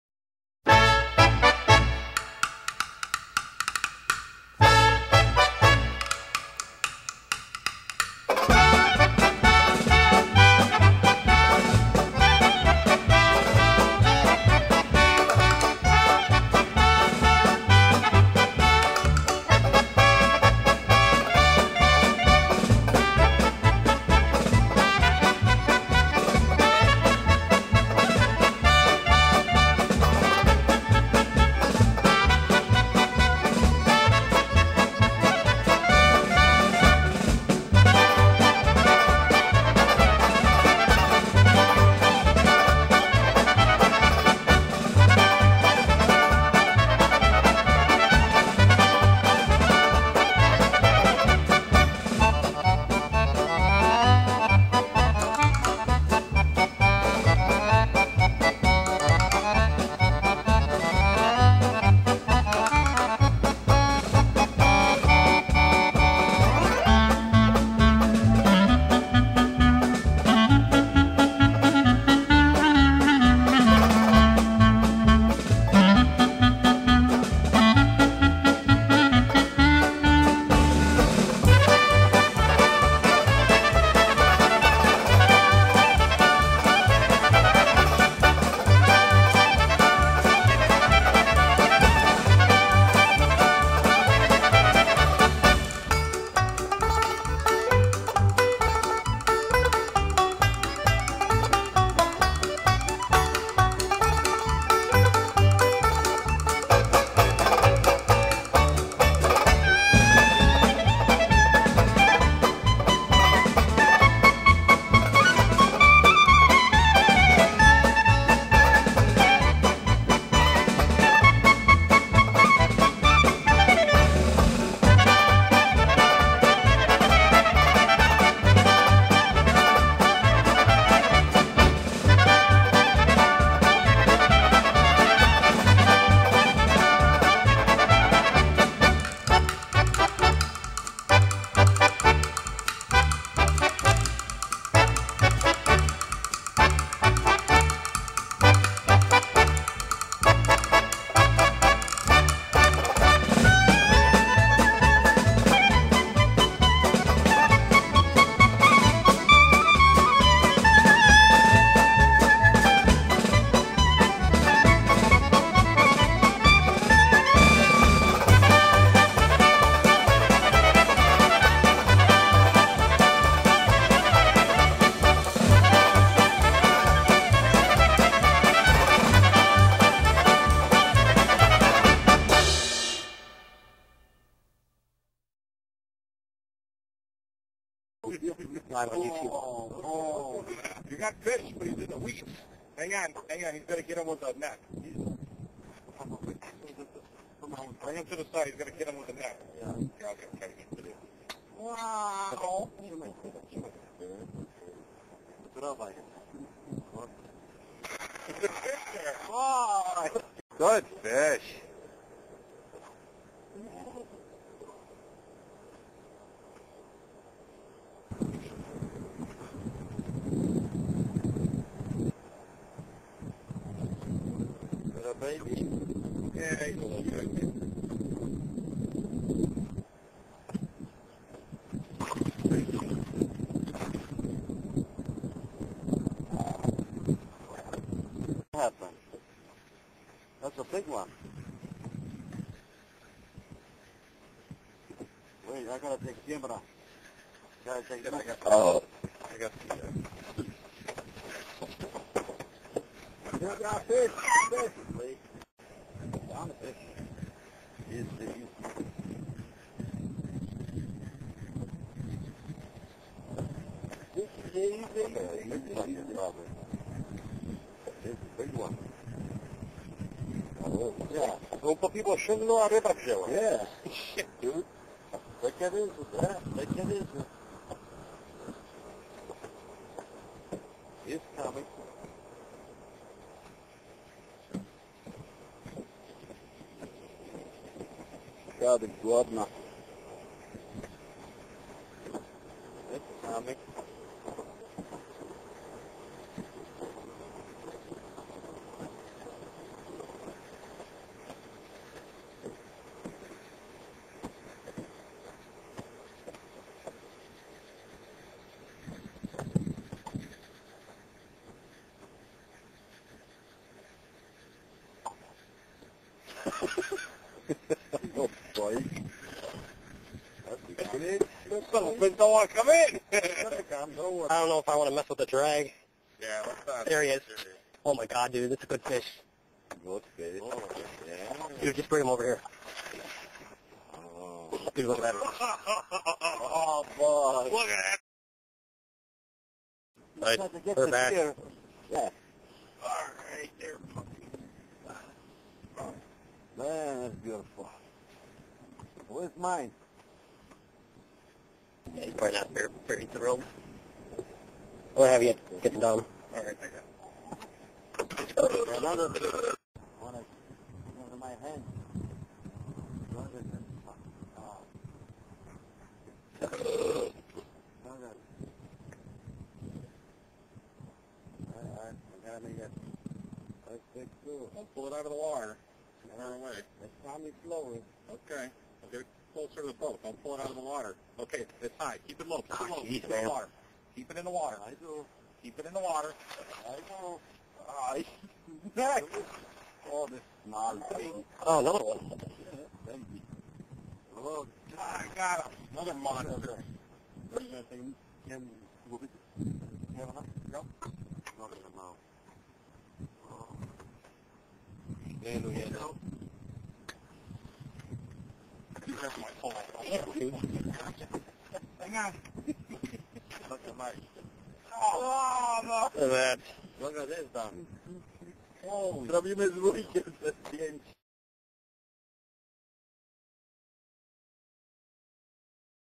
Us Pollacks can fish !! -- orlandotrophybass.com
I had a great time fishing with my long time friend Don Trzepacz from Buffalo, NY. Along with his father-in-law, and his father-in-laws brother they are fresh off the boat from the motherland Poland. The fish they caught should put an end to picking on us polish folks.